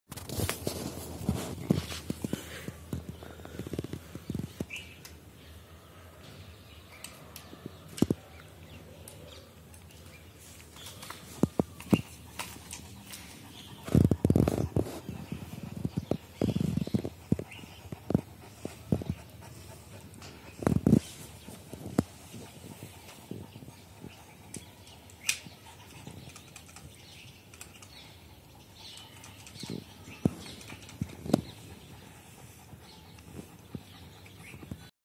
Tibetan Mastiff Shedding
When You own a dog this big there are some responsibilities and drawbacks too.
One of them is Shedding,which you can see in the video above

WE USE 2 BRUSHES/COMB FOR SHEDDING! HERE ARE THE LINKS FOR THE SAME 👇🏼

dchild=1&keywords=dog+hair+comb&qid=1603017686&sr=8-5